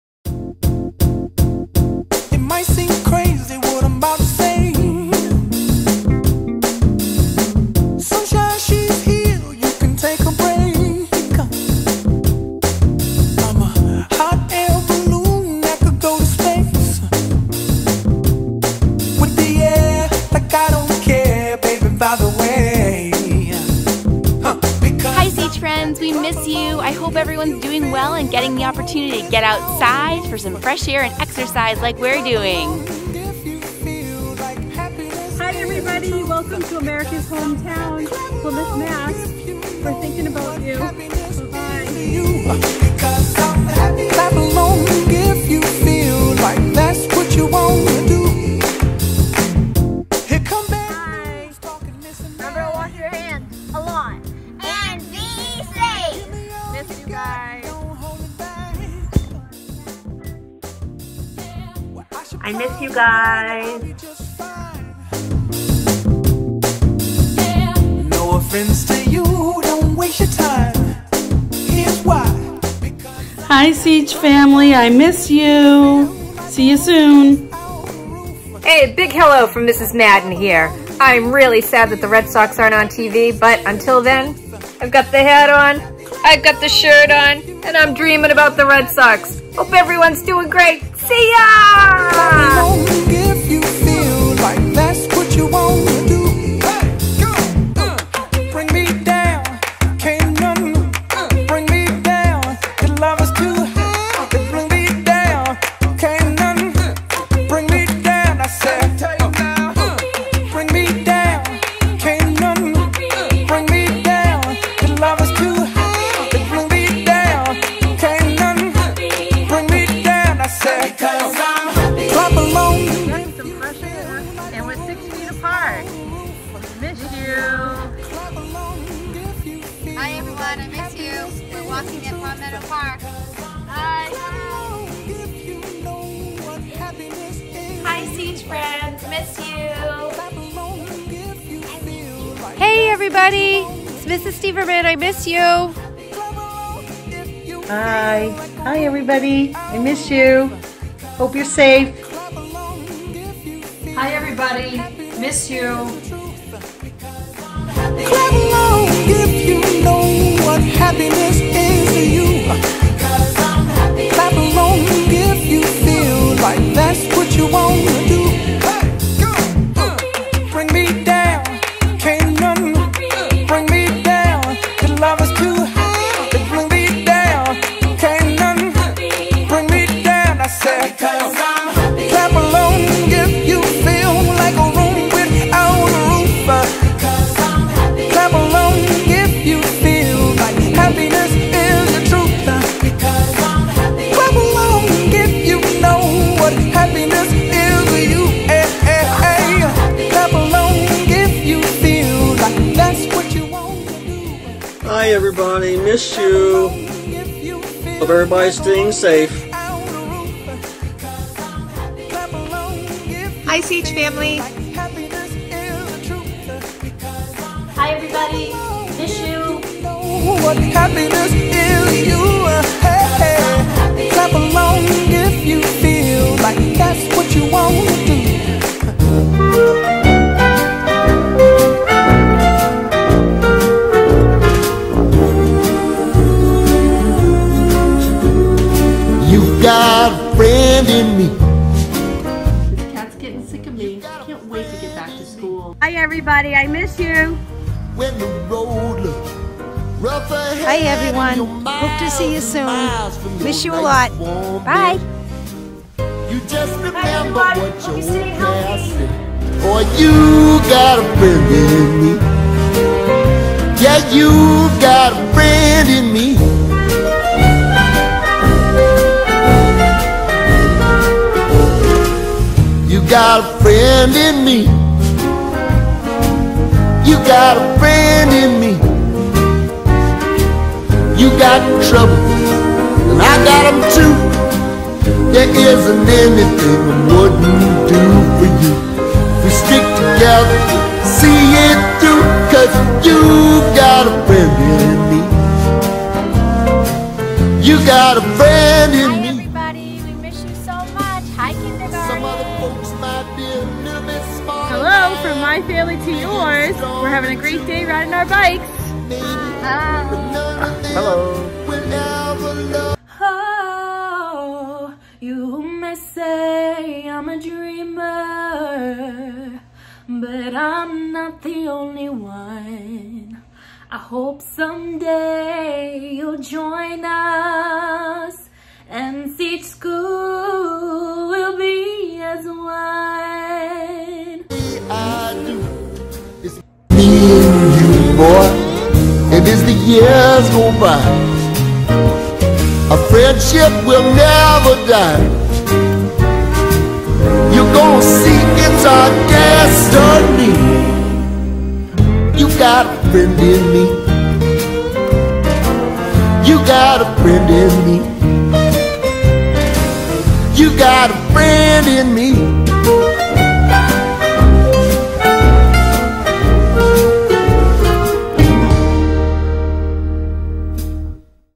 Seach School Hello!
Hello from the William Seach Primary School staff and families!